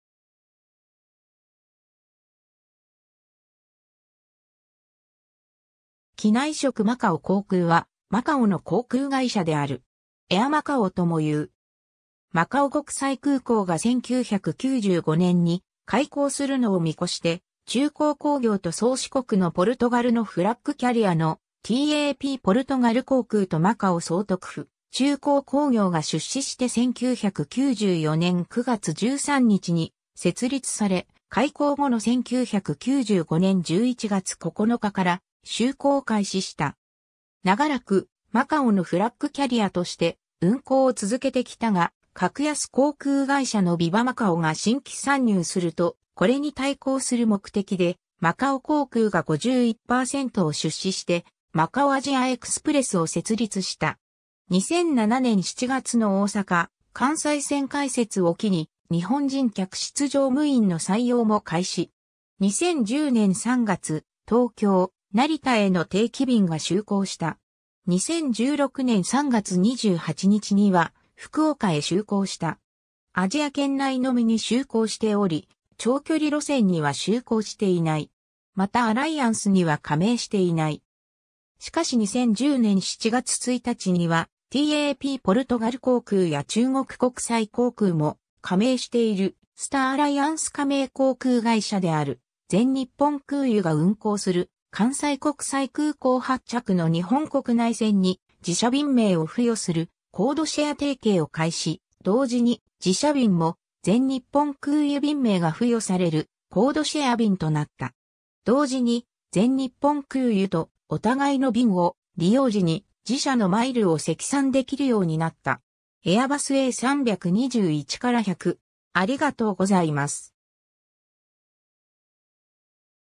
マカオ航空
機内食 マカオ航空（マカオこうくう）は、マカオの航空会社である。
エア・マカオとも言う。
 マカオ国際空港が1995年に開港するのを見越して、中航興業と宗主国のポルトガルのフラッグ・キャリアのTAP ポルトガル航空とマカオ総督府（マカオの中華人民共和国返還後は特別行政区政府）、中航興業が出資して1994年9月13日に設立され、開港後の1995年11月9日から就航開始した。
 長らくマカオのフラッグ・キャリアとして運航を続けてきたが、格安航空会社のビバ・マカオ（後に倒産）が新規参入すると、これに対抗する目的でマカオ航空が51%を出資してマカオ・アジア・エクスプレスを設立した（運航を開始することなく後に清算）。
 2007年7月の大阪/関西線開設を機に、日本人客室乗務員の採用も開始。
2010年3月、東京/成田への定期便が就航した。
2016年3月28日には福岡へ就航した。
 アジア圏内のみに就航しており、長距離路線には就航していない。
またアライアンスには加盟していない。
しかし2010年7月1日には、TAPポルトガル航空や中国国際航空も加盟しているスターアライアンス加盟航空会社である全日本空輸が運航する関西国際空港発着の日本国内線に、自社便名を付与するコードシェア提携を開始、同時に自社便も全日本空輸便名が付与されるコードシェア便となった。
同時に、全日本空輸とお互いの便を利用時に自社のマイルを積算できるようになった。
 エアバスA321-100